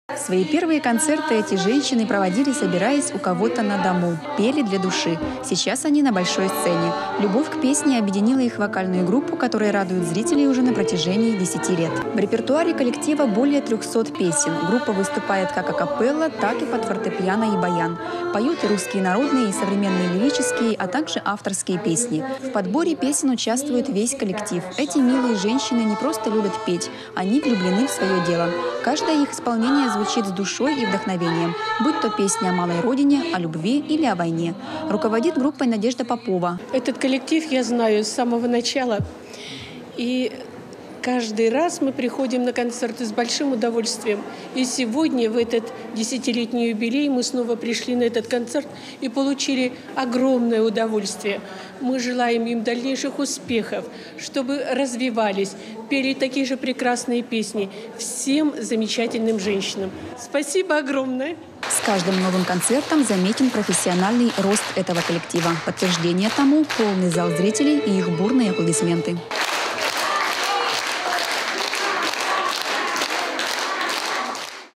Новодвинская "Надежда", ансамбль песни
Их объединила любовь к песне. Праздничным концертом отметили 10-летний юбилей участники вокальной группы "Надежда". Впечатлениями от праздника поделились наши коллеги из Новодвинска.